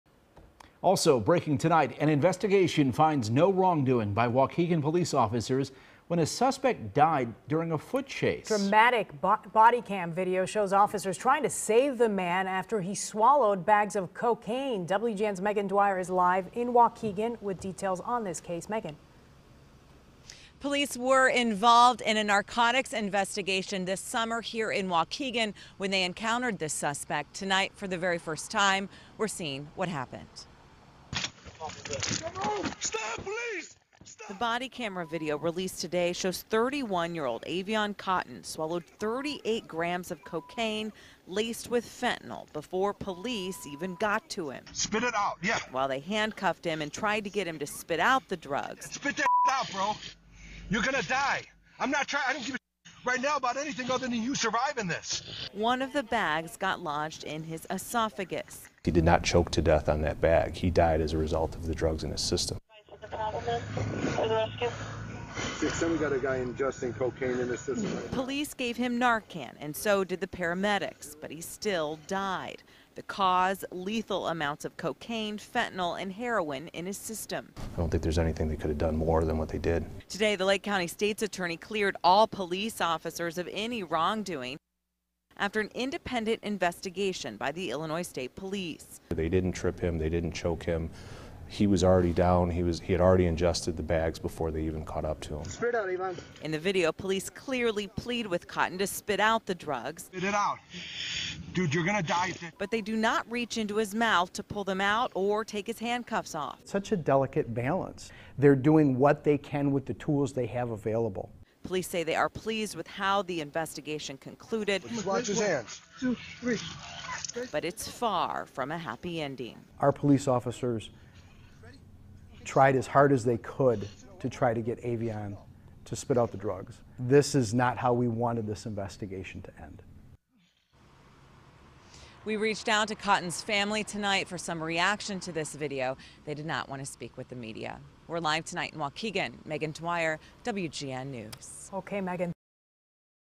Waukegan officers cleared of wrongdoing following in-custody death last summer
The Lake County State`s Attorney`s Office found no wrongdoing by Waukegan police in the "in-custody" death of a man last summer.